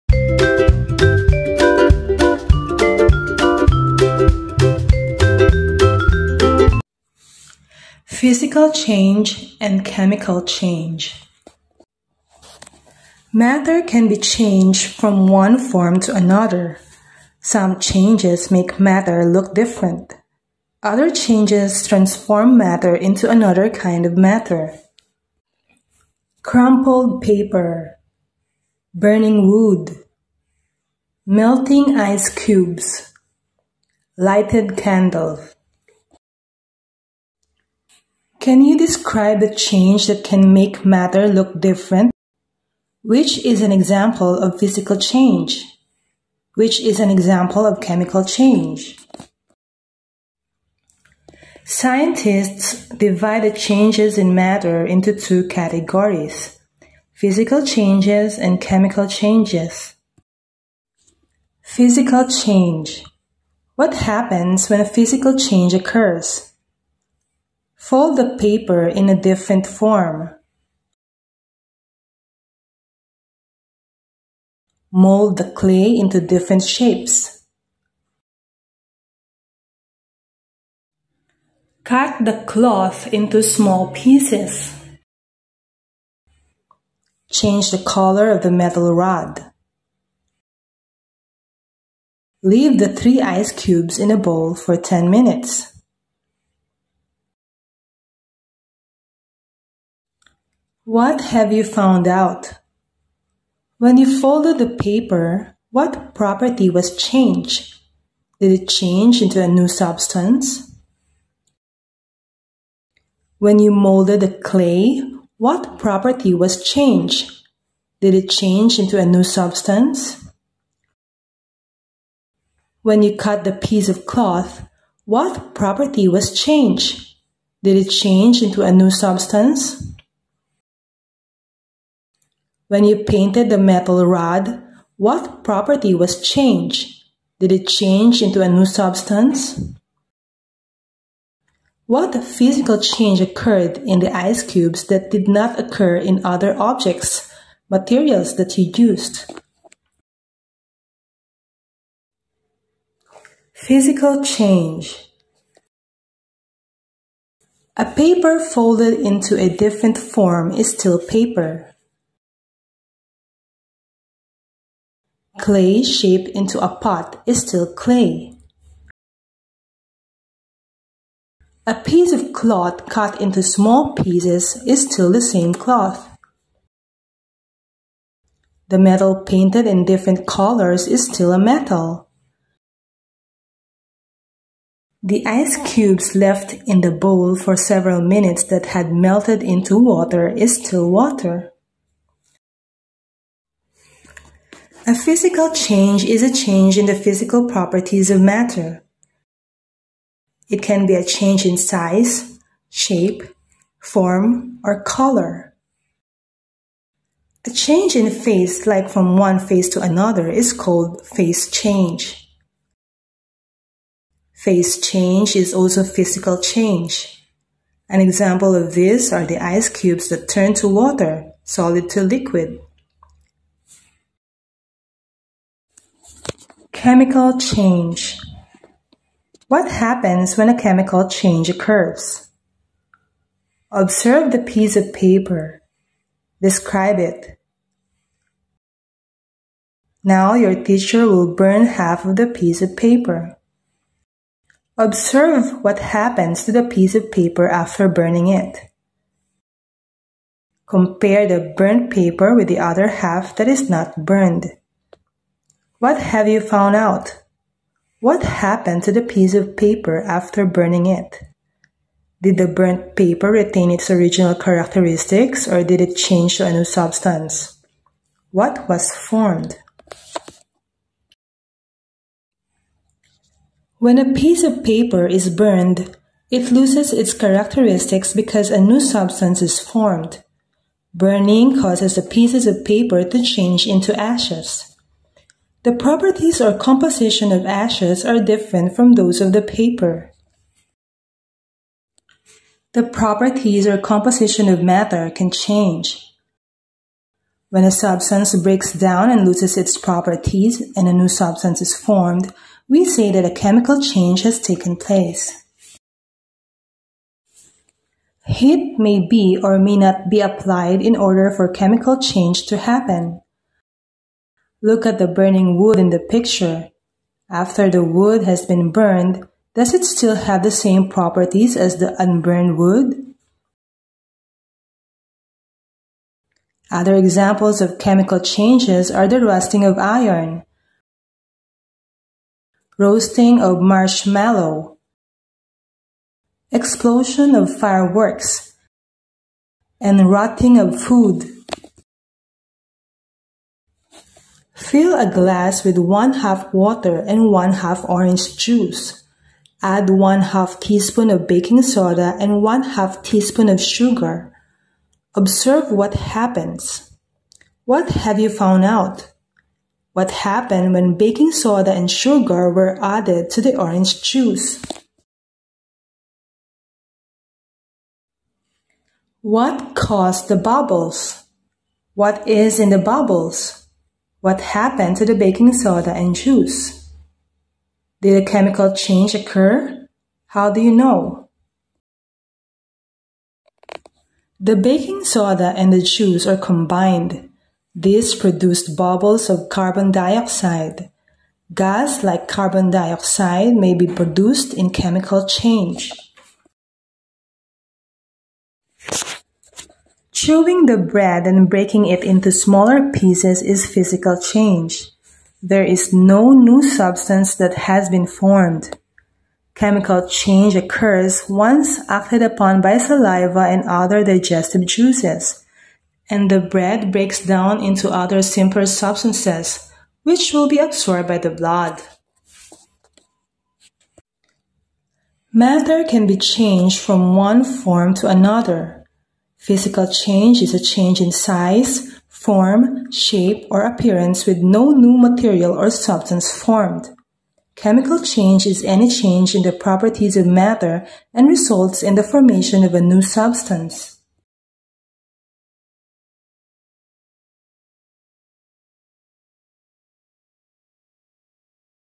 PHYSICAL CHANGE AND CHEMICAL CHANGE / CLEVER SCHOOL PH
Differentiate physical change from chemical change. Demonstrate ways on how matter undergoes changes in properties.